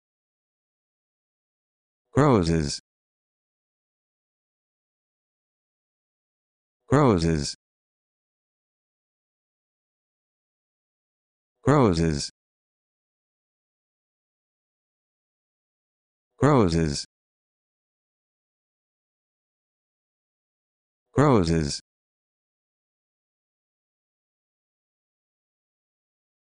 How to Pronounce Crozes
This video shows you how to pronounce Crozes